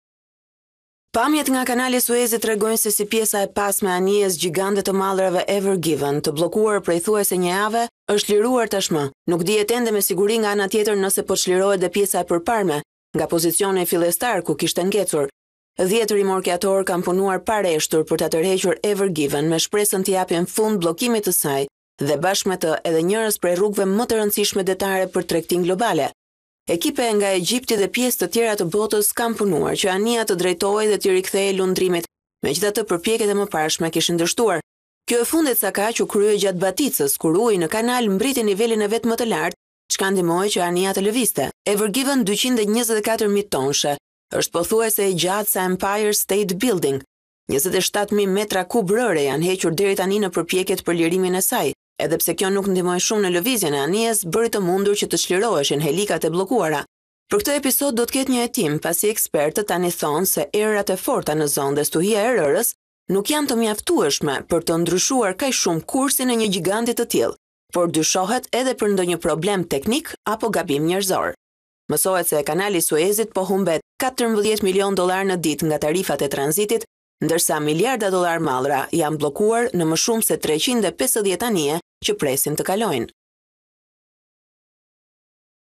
Top News - Shpresë në kanalin e Suezit/ Zhbllokohet pjesërisht anija pas thuajse një jave
Pamjet filmike nga kanali I Suezit tregojnë sesi pjesa e pasme e anijes gjigande të mallrave, Ever Given, të bllokuar prej thuajse një jave, është zhbllokuar dhe është larguar nga njëri breg. Nuk dihet ende me siguri nga ana tjetër nëse pjesa e përparme po clirohet nga pozicioni fillestar ku kishte ngecur. 10 rimorkiatorë kanë punuar pa reshtur për ta tërhequr Ever Given me shpresën t`I japing fund bllokimit të saj dhe njërës prej rrugëve më të rëndësishme detare për tregtinë globale. Ekipe nga Egjipti dhe pjesë të tjera të botës kanë punuar pa reshtur që anija të drejtohej e t`I rikthehej lundrimit, por përpjekjet e mëparshme kishin dështuar. Kjo e fundit sakaq, u krye gjatë baticës kur uji në kanal mbërrin nivelin e vet më të lartë, cka ndihmoi që anija të lëvizte. Ever given, 224 mijë tonë është pothuajse e gjatë sa Empire State Building. 27 mijë metra kub rërë janë hequr deri tani në përpjekjet për lirimin e saj. Edhe pse nuk ndihmoi shumë në lëvizjen e anijes, kjo bëri që të cliroheshin helikat e bllokuara pas hasjes në cektinë. Për këtë episode do të ketë një hetim, pasi ekspertët tani thotë që erërat e forta në zonë dhe stuhia e rërës nuk janë të mjatueshme për të ndryshuar kaq fort kursin e një gjigandi të tillë, por dyshohet edhe për ndonjë problem teknik apo gabim njerëzor. Mësohet se kanali I Suezit po humbet 14  milionë dollarë në ditë nga tarifat e tranzitit, ndërsa miliarda dollare mallra janë bllokuar në më shumë se 350 anije që presin të kalojnë.

Pamje: AFP